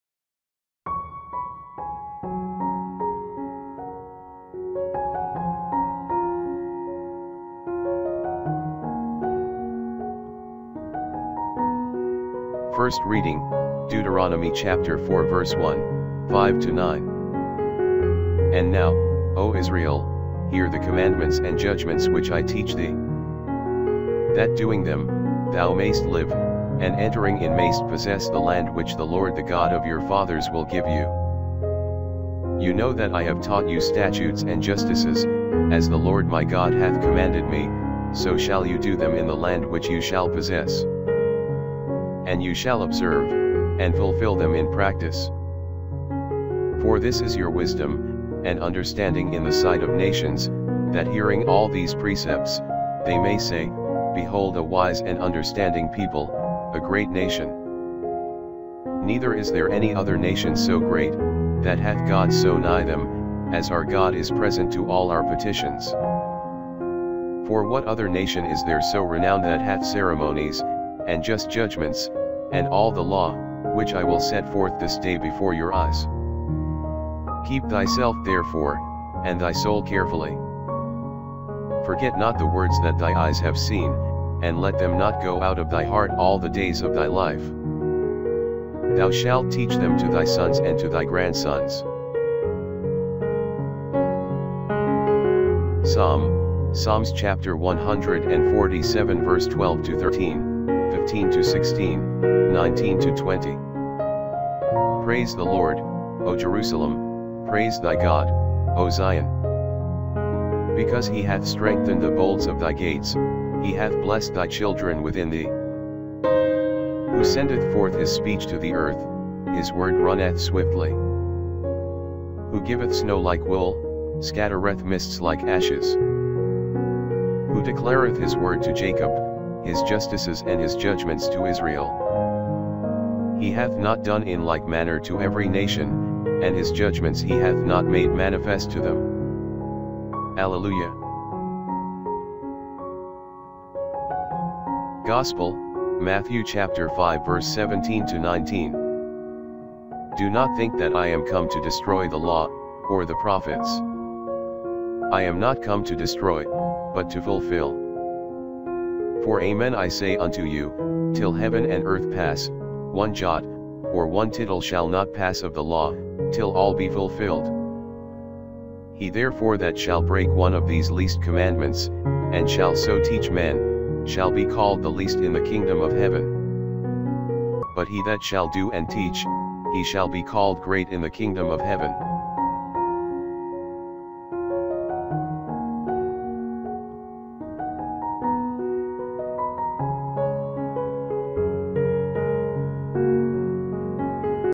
DAILY BIBLE READING - MARCH 18, 2020
THIS IS THE WORD OF GOD FOR THE DAY. PLEASE TAKE TIME TO LISTEN AND REFLECT. LET US BRING THE WORD OF GOD AMONG ALL PEOPLE.

GOD BLESS!

First Reading: Deuteronomy 4:1, 5-9
1 And now, O Israel, hear the commandments and judgments which I teach thee: that doing them, thou mayst live, and entering in mayst possess the land which the Lord the God of your fathers will give you.
5 You know that I have taught you statutes and justices, as the Lord my God hath commanded me: so shall you do them in the land which you shall possess:
6 And you shall observe, and fulfill them in practice. For this is your wisdom, and understanding in the sight of nations, that hearing all these precepts, they may say: Behold a wise and understanding people, a great nation.
7 Neither is there any other nation so great, that hath gods so nigh them, as our God is present to all our petitions.
8 For what other nation is there so renowned that hath ceremonies, and just judgments, and all the law, which I will set forth this day before your eyes?
9 Keep thyself therefore, and thy soul carefully. Forget not the words that thy eyes have seen, and let them not go out of thy heart all the days of thy life. Thou shalt teach them to thy sons and to thy grandsons,

12 Praise the Lord, O Jerusalem: praise thy God, O Sion.
13 Because he hath strengthened the bolts of thy gates, he hath blessed thy children within thee.
15 Who sendeth forth his speech to the earth: his word runneth swiftly.
16 Who giveth snow like wool: scattereth mists like ashes.
19 Who declareth his word to Jacob: his justices and his judgments to Israel.
20 He hath not done in like manner to every nation: and his judgments he hath not made manifest to them. Alleluia.

17 Do not think that I am come to destroy the law, or the prophets. I am not come to destroy, but to fulfill.
18 For amen I say unto you, till heaven and earth pass, one jot, or one tittle shall not pass of the law, till all be fulfilled.
19 He therefore that shall break one of these least commandments, and shall so teach men, shall be called the least in the kingdom of heaven. But he that shall do and teach, he shall be called great in the kingdom of heaven.